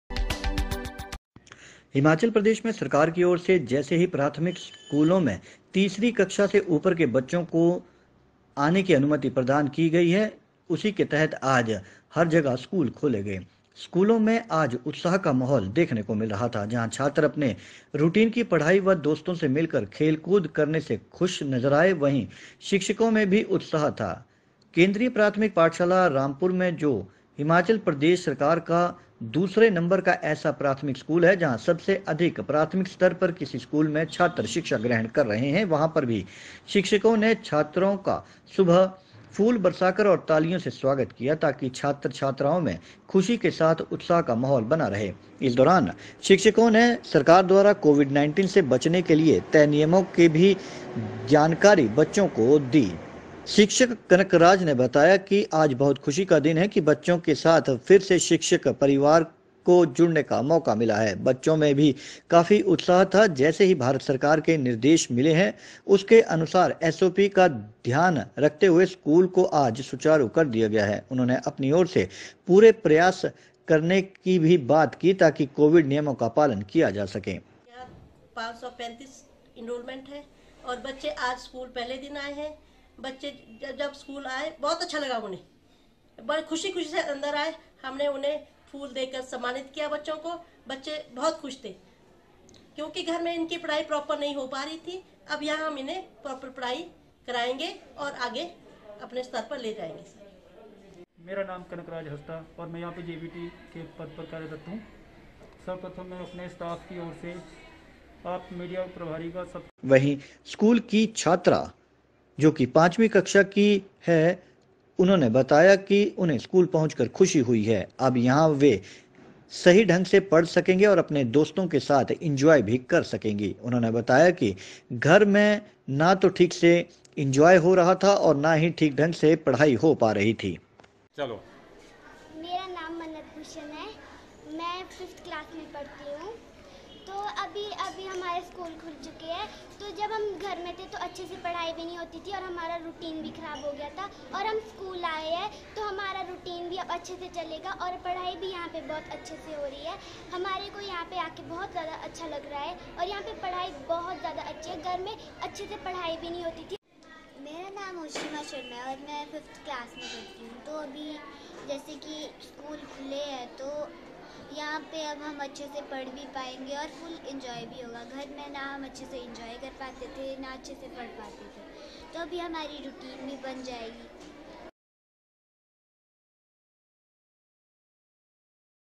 rampur केंद्रीय प्राथमिक पाठशाला रामपुर में बच्चों का प्रगमन पर फूलों से किया गया स्वागत
करीब पौने दो साल बाद छोटी कक्षाओं के बच्चों के लिए  स्कूल
खुलते ही बच्चों में दिख उत्साह।  शिक्षकों ने ने भी छात्रों का किया
जोरदार स्वागत। सब से अधिक  इनरोलमेंट करने वाले हिमाचल के दूसरे नंबर के
 स्कूल  केंद्रीय प्राथमिक पाठशाला  रामपुर में बच्चों का प्रथम दिन
स्कूल आगमन पर फूलों से किया गया स्वागत।  शिक्षकों ने कहा बच्चों में
उत्साह एवं सकारात्मक सोच तैयार करने के लिए उनका किया गया स्वागत।